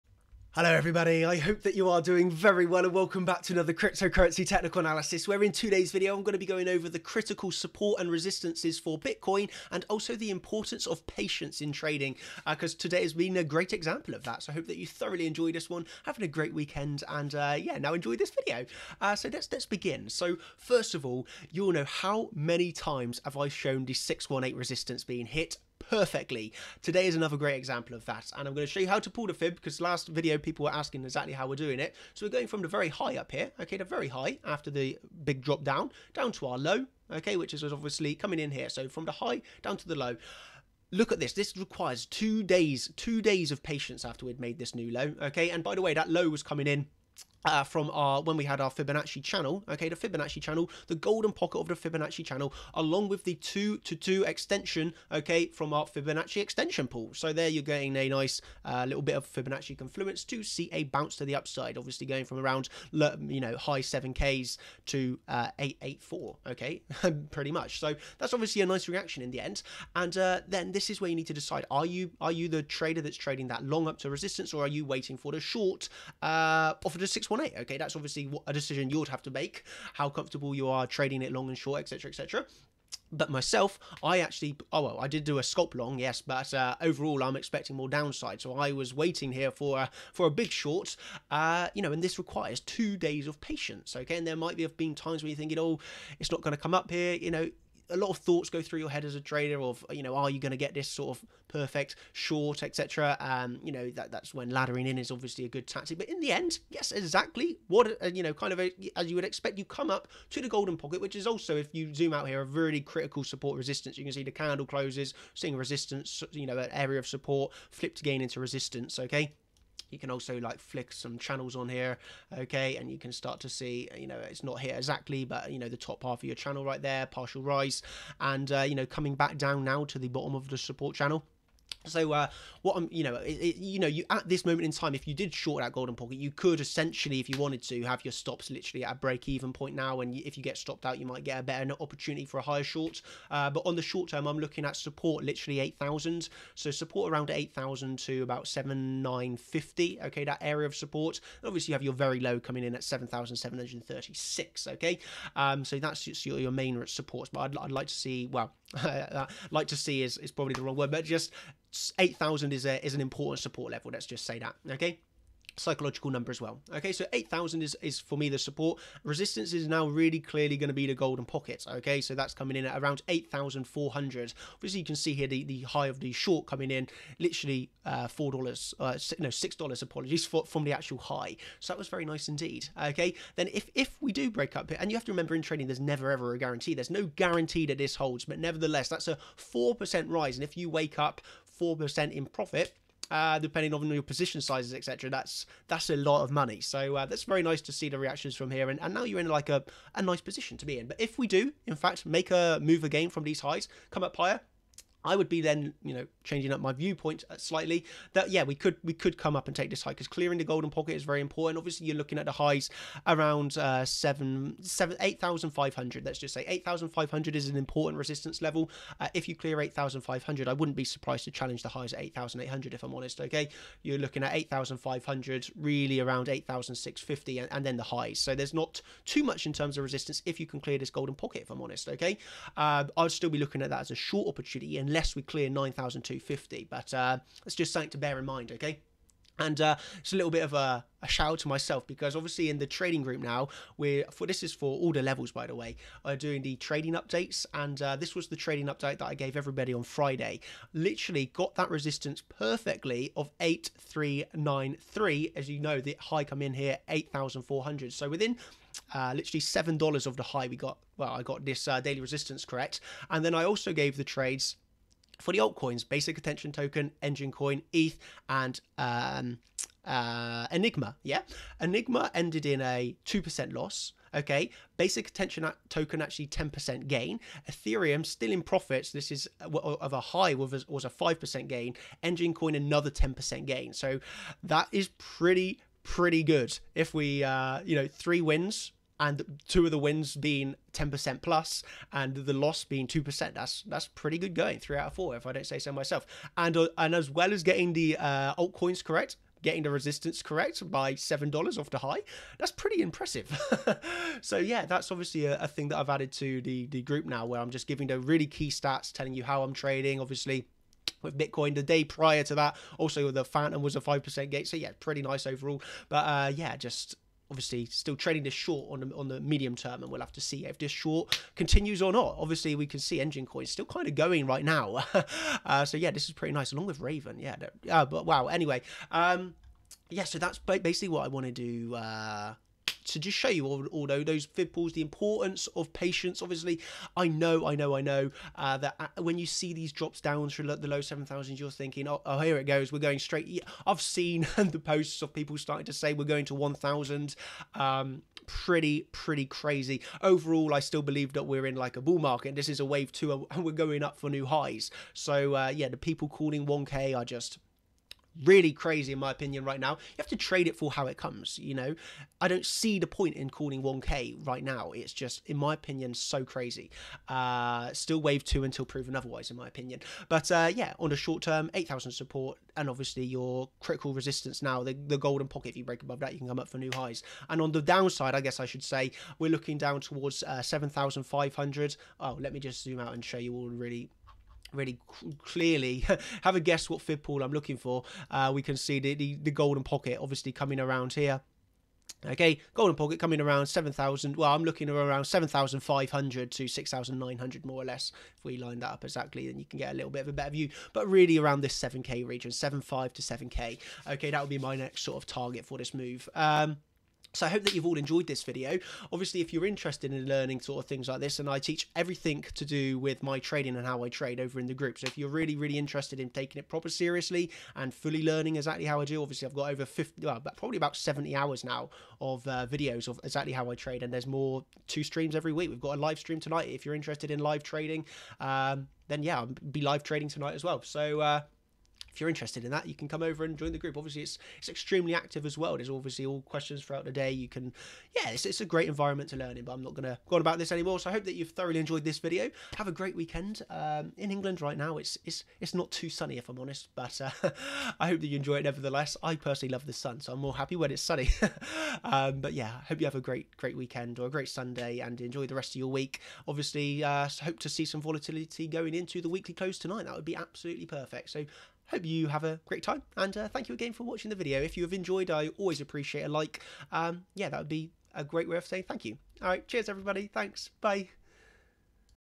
Fibonacci trading perfectly. BTC - Bitcoin Technical Analysis.
BTC - Bitcoin Technical Analysis price today 29.9.19 Bitcoin is held up by the golden pocket resistance once again, but bulls are currently holding 8000 support, but will it last?